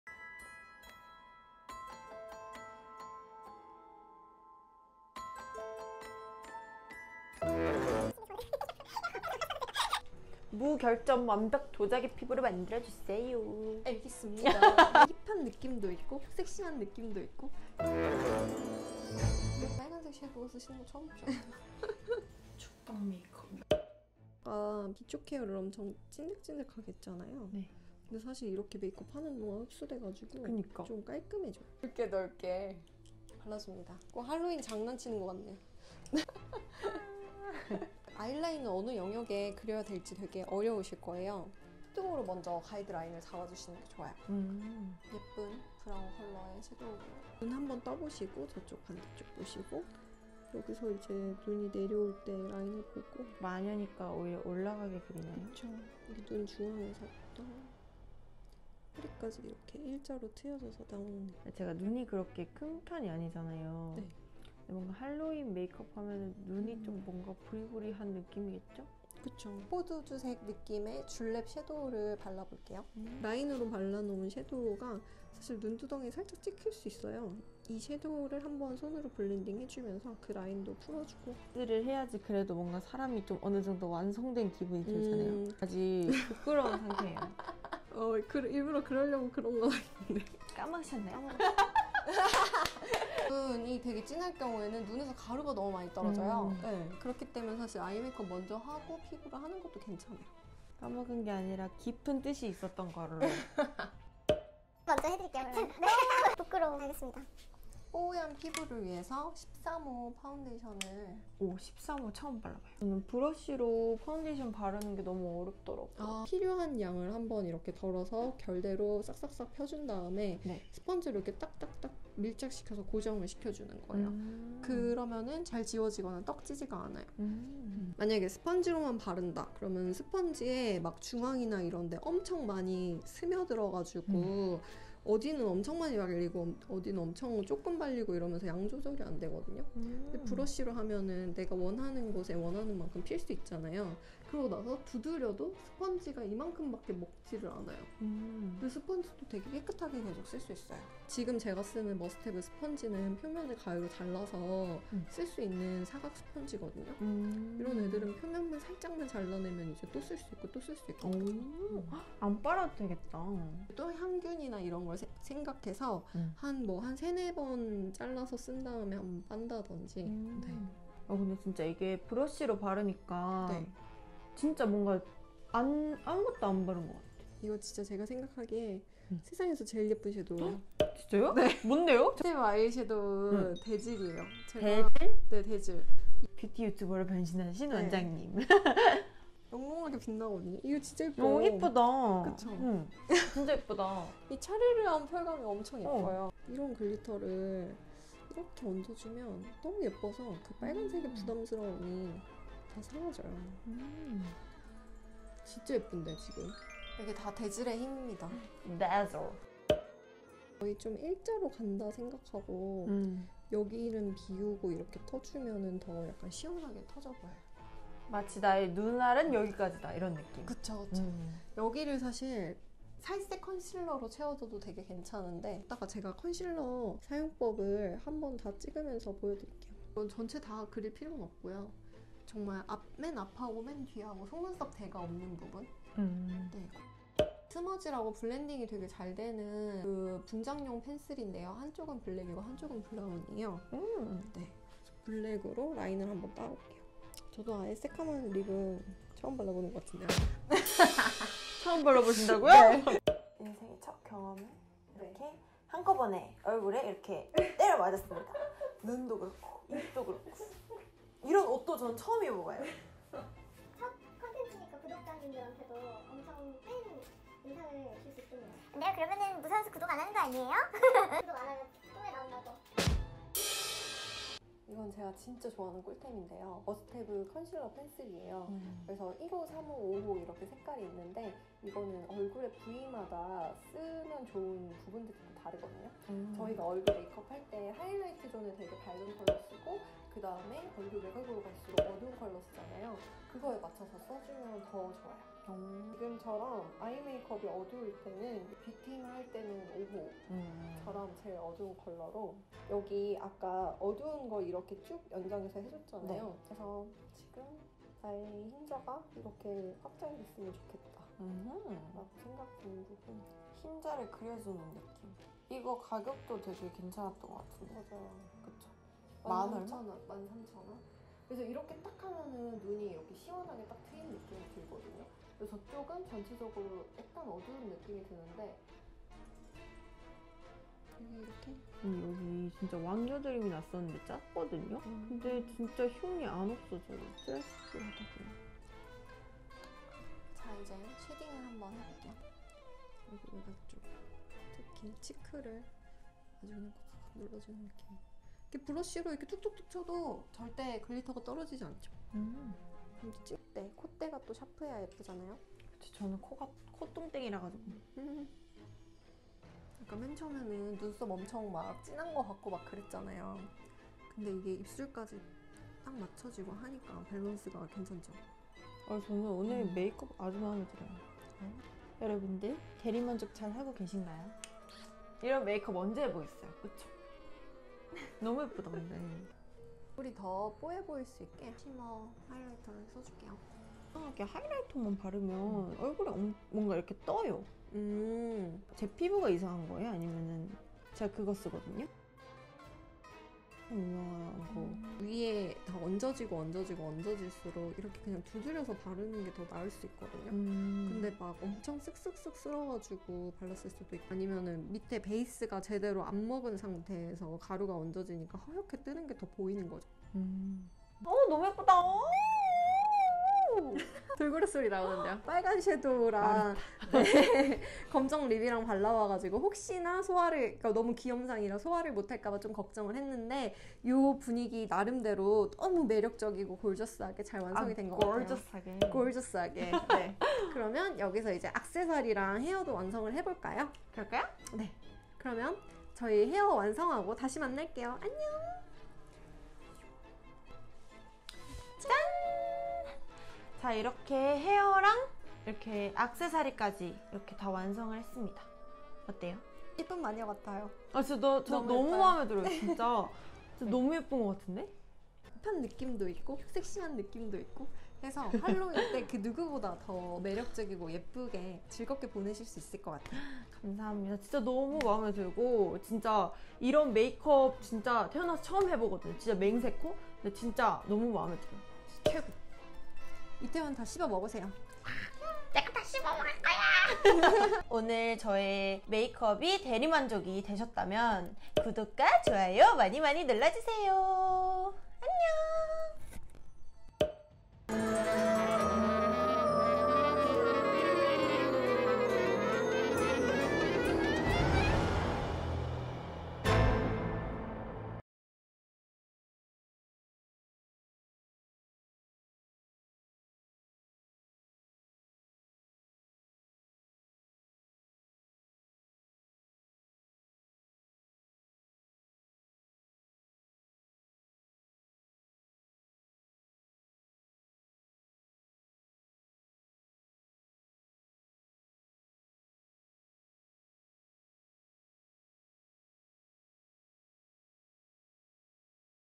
[머스테브 출연 클립 모음] 할로윈 특집! 이태원 씹어먹는 매력 마녀 메이크업! 뷰대리 섹시 파티 메이크업 코스프레
너무나 유쾌한 뷰대리 채널에서
머스테브 제품들을 활용해 할로윈 메이크업을 보여주셨어요 💕

담당자 입장에서 뷰대리님 채널을 통해 너무나 행복하게 대리만족 되었습니다 ❤️

전체 제품을 볼 수 있는 풀 영상은 뷰대리 채널에서 확인해주세요

[ 아이메이크업 1]

머스테브 아이섀도우 - 팝핀레드
머스테브 시크릿 브러시 - 패팅 섀도우
머스테브 아이섀도우 - 스파클나잇
머스테브 시크릿 브러쉬 - 탤런트
머스테브 아이섀도우 - 쥴렙
머스테브 시크릿 브러쉬 - 패팅 섀도우

[ 베이스 1 ]

머스테브 스키니 틴트 파운데이션 이노센트
머스테브 시크릿 브러쉬 - 파운데이션
머스테브 프로 뷰티 스펀지 블럭

[ 아이메이크업 2 ]

머스테브 시크릿 브러쉬 - 트윙클 아이
머스테브 아이섀도우 - 대즐
머스테브 텐션 핏 리퀴드 라이너 브러쉬 펜 - 블랙

[ 립, 컨투어링 ]
머스테브 스튜디오 멀티 유즈 펜슬 - 블랙
머스테브 스팟 이레이저 컨실러 펜슬 - 5호 뉴트럴 베이지
머스테브 스팟 이레이저 컨실러 펜슬 - 3호 라이트 베이지
머스테브 페이스 아키텍트 파우더 - 실루엣
머스테브 고원혜 브러쉬 - 02 쉐이딩
머스테브 고원혜 브러쉬 - 10 라지 섀도우
머스테브 페이스 아키텍트 파우더 - 라이트온
머스테브 고원혜 브러쉬 - 05 하이라이터